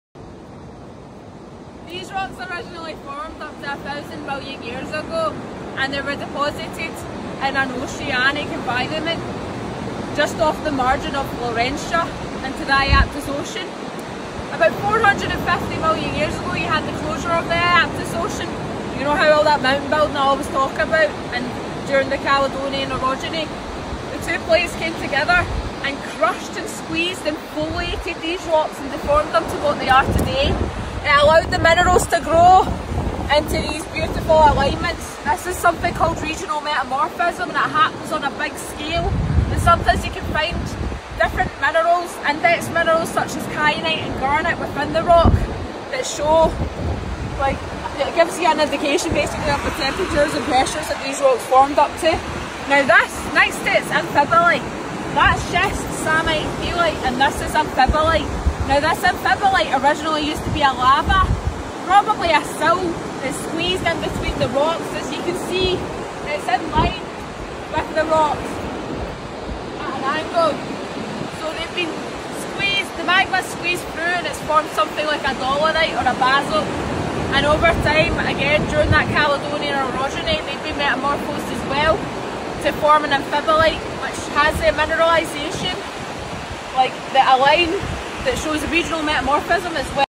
The geology of the Isle of Mull, Ardlanish bay
The geology of the Isle of Mull, Ardlanish Bay - The Moine supergroup. Find yourself a girl that loves you like she loves these rocks 😂.